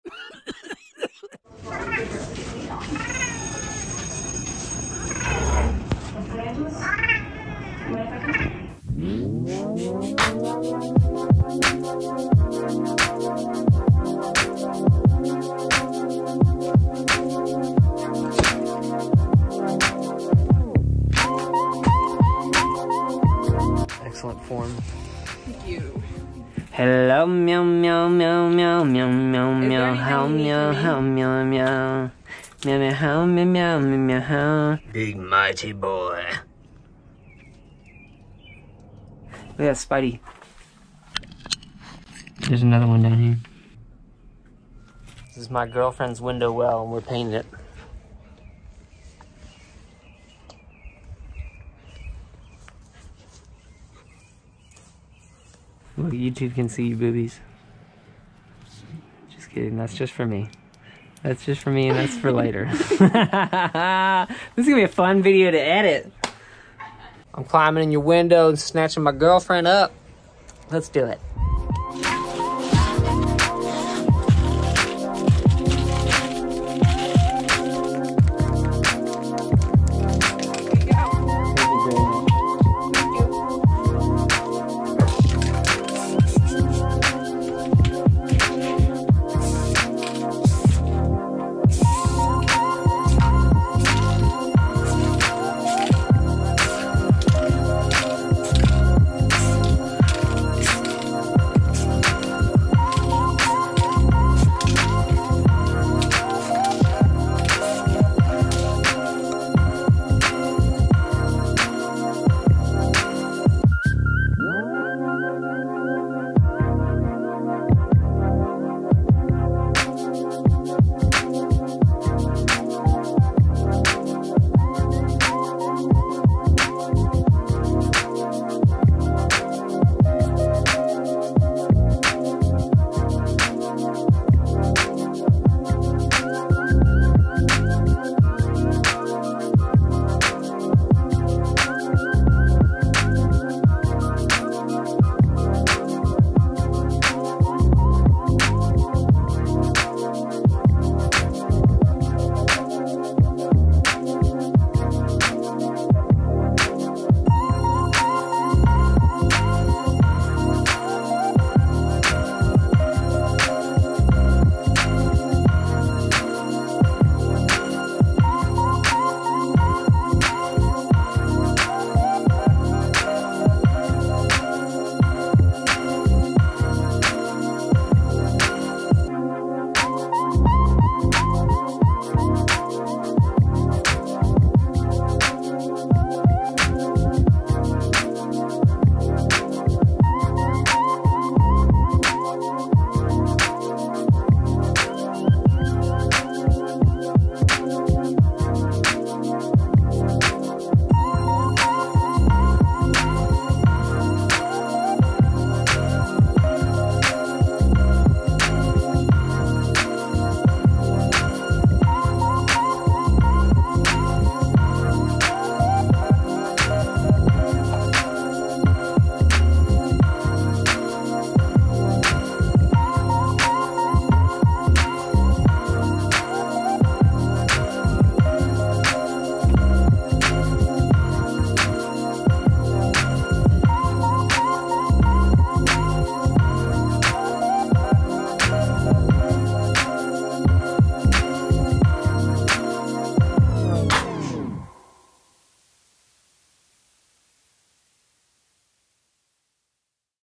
I violated HOA regulations by painting on her MOM'S HOUSE
Don't tell HOA!! i painted this cute troll in the window well of my girlfriends room.  What do you think?